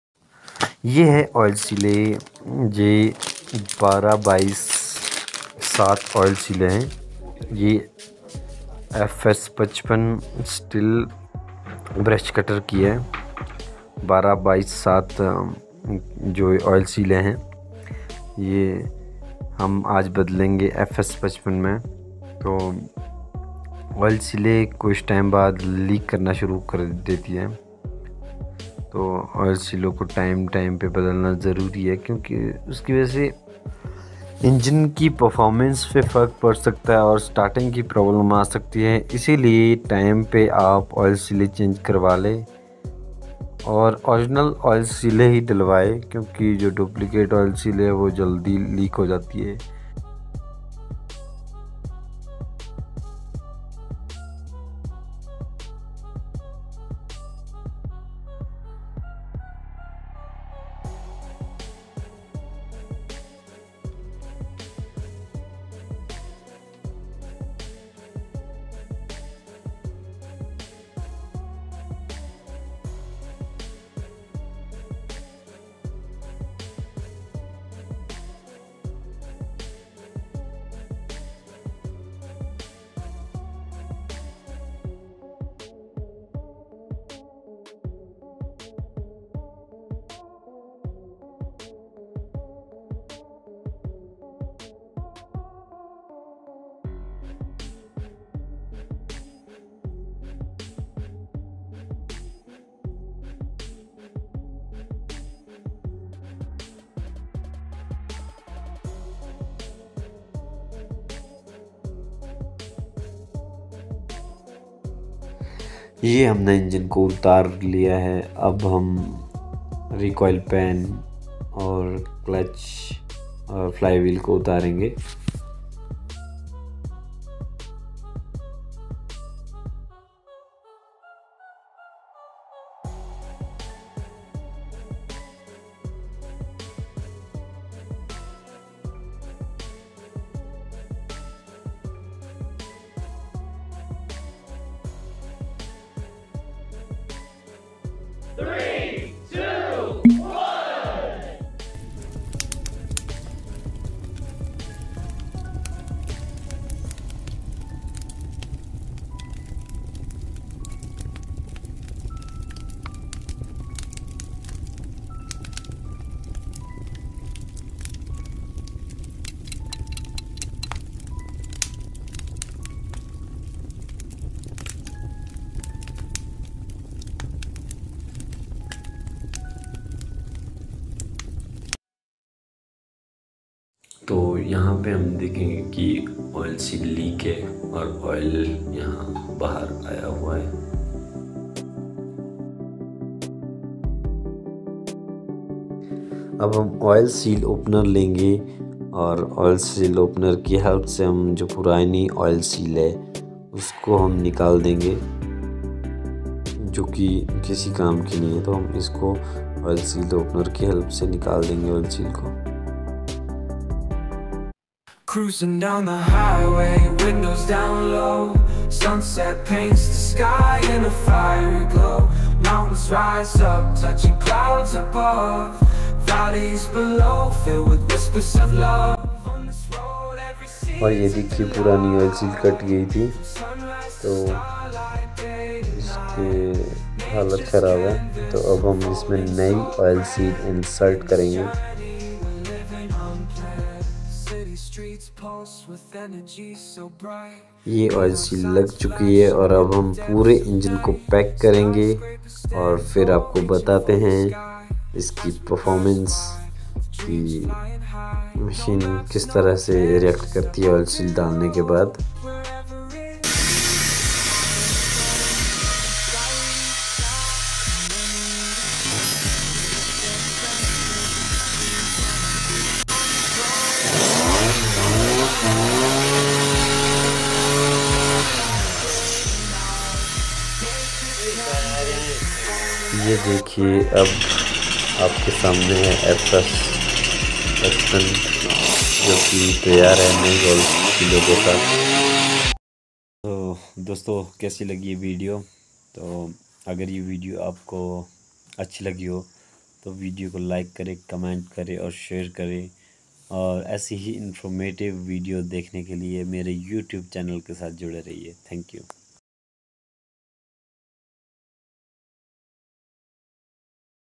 How to change a oil seal in FS 55 STIHL brush cutter ⚙️stihl
How to change a oil seal  in FS55 STIHL brush cutter
Today, I’m going to show you how to change the oil in your STIHL FS55 brush cutter. Regular maintenance is key to keeping your equipment running smoothly, so let’s get started!”

Step 1: Gather Your Supplies "First, you’ll need a few things:

A clean cloth
A container for the old oil
Fresh oil (check your manual for the recommended type)
A funnel
A wrench"
Step 2: Prepare Your Brush Cutter “Make sure your brush cutter is turned off and cool to the touch. Place it on a flat surface and remove any debris around the oil cap.”

Step 3: Drain the Old Oil “Locate the oil cap on the engine. Use your wrench to carefully unscrew the cap. Tilt the brush cutter to drain the old oil into your container. Be sure to dispose of the old oil properly.”

Step 4: Add New Oil “Once the old oil is completely drained, use your funnel to add the new oil. Pour slowly to avoid spills. Check the oil level with the dipstick to ensure you have the right amount.”

Step 5: Reassemble and Clean Up “Screw the oil cap back on securely. Wipe down any spills with your clean cloth. Make sure everything is tight and in place.”

Conclusion: “And that’s it! You’ve successfully changed the oil in your STIHL FS55 brush cutter. Regular maintenance like this will help extend the life of your equipment.